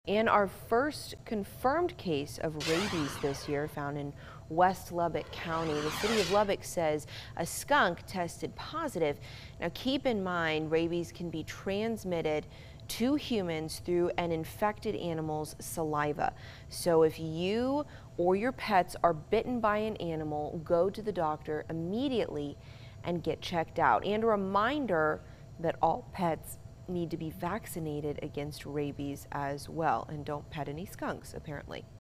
Rabies confirmed in Lubbock County
A skunk that was found in western Lubbock County tested positive for rabies, the City of Lubbock said on Monday.